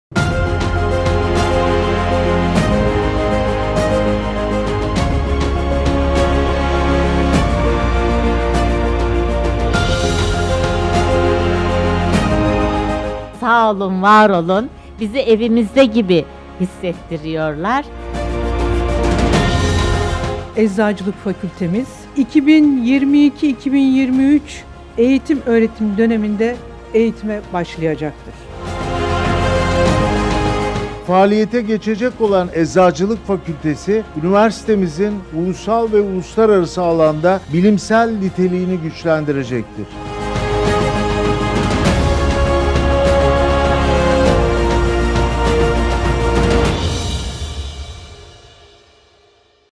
VI. Ulusal Farmasötik Kimya Kongresi
Fenerbahçe Üniversitesi ve Farmasötik Kimya Derneği tarafından ortaklaşa düzenlenen “VI. Ulusal Farmasötik Kimya Kongresi” Eczacılık Fakültemizin ev sahipliğinde, Sayın Rektörümüz Prof. Dr. M. Emin Arat, Eczacılık Fakültesi Dekanımız Prof. Dr. Güniz Küçükgüzel, değerli hocalarımız ve konuklarımızın katılımlarıyla gerçekleştirildi.